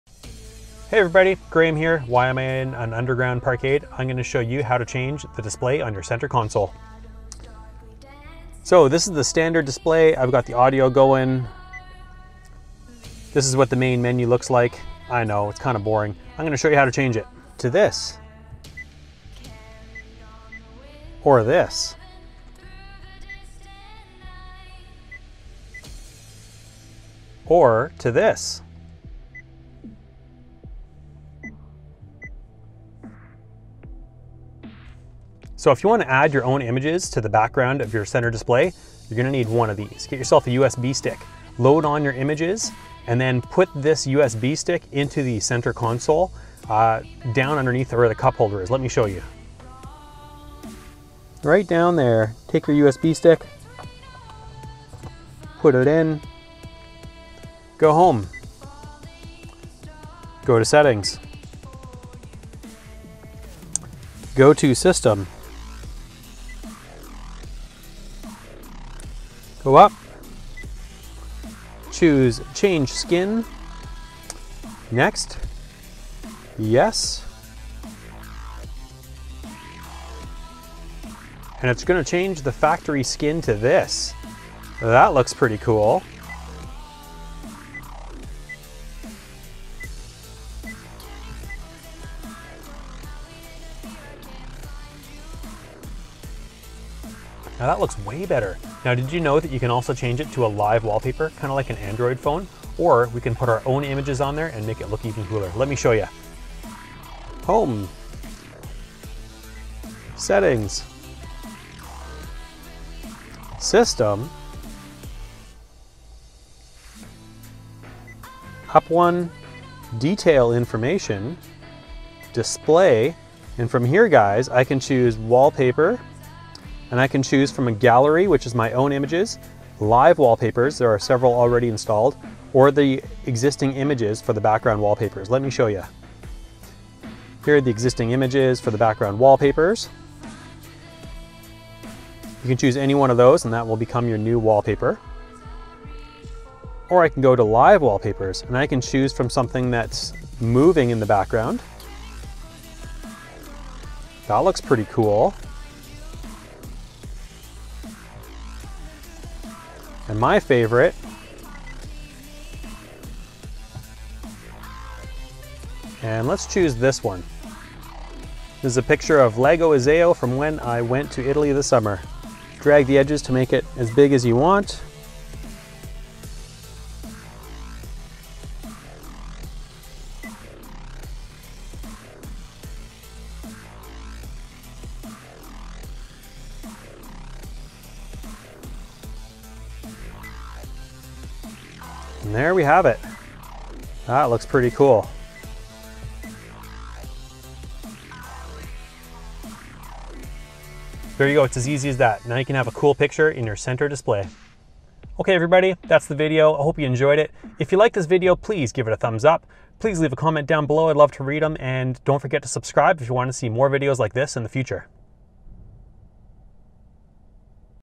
HOW TO CHANGE HONDA CIVIC CENTER CONSOLE DISPLAY (2016-2017)
If you find the current center display audio console on the new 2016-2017 Honda Civic a little boring, then you are not alone.  The good news is that we can easily change the center console display on the new 10th generation Honda Civic.

A quick guide to show you the options, which include changing the skin on the audio and center display, choosing from one of the pre-loaded images for the background wallpaper on the touchscreen, selecting a live wallpaper for your Honda Civic touch screen, or loading one of your own pictures.

The Honda Civic touch screen will look a lot better once you choose the right background for the center display console, and is a free way to customize your new 2017 Honda Civic to match your personal style.